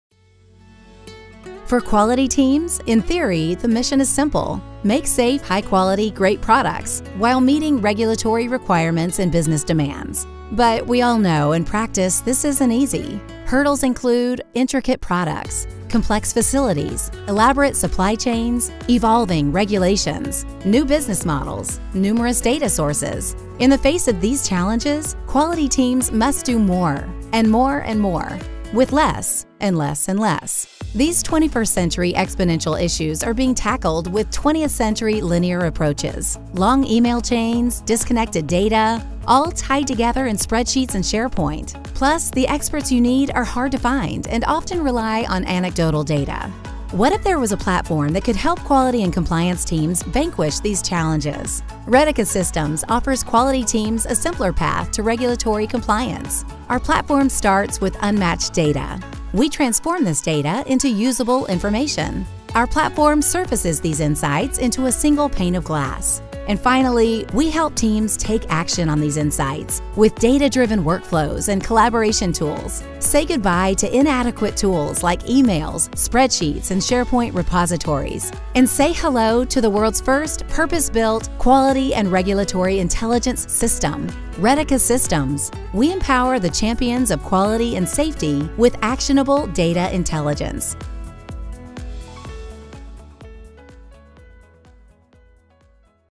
Redica Systems
Redica Systems empowers the champions of quality and safety with actionable data intelligence.

We are the first quality and regulatory intelligence (QRI) platform for the pharma, biopharma, medical device, and food and cosmetics industries.

Our proprietary processes transform millions of data points and signals, from hundreds of health agencies, into meaningful answers and insights that reduce regulatory and compliance risk.